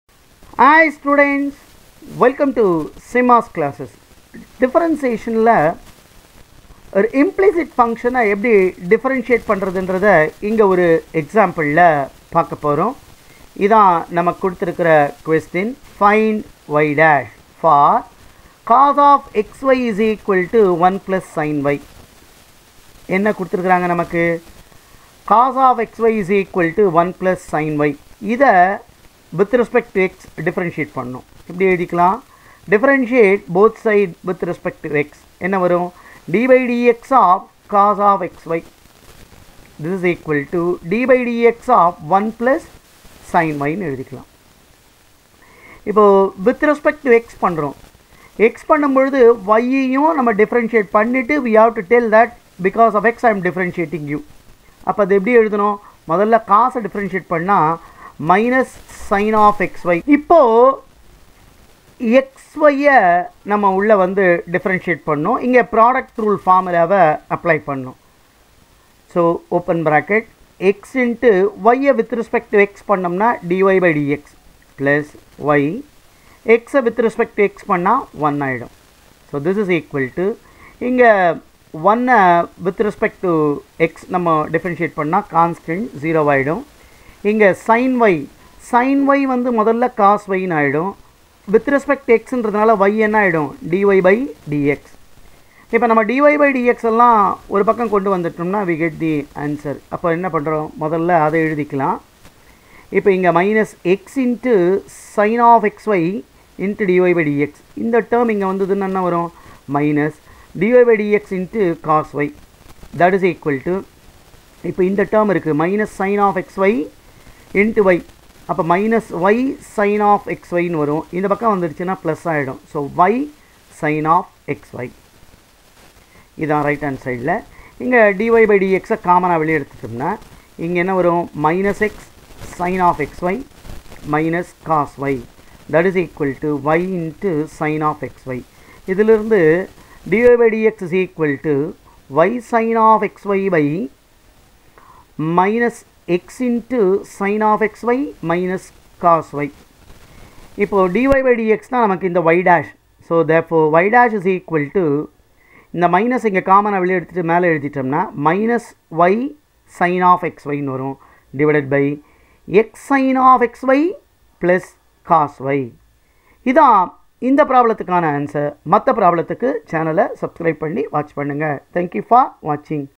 MA3151 - PART B - UNIT 2 - Differentiation - Implicit function - Find y' for cos(xy)=1+siny
MA3151, Matrices and Calculus - QP Code 70132 Anna University Question Paper  NOV DEC 22
MA3151, Matrices and Calculus - QP Code 60043 Anna University Question Paper  APR MAY 22 - solved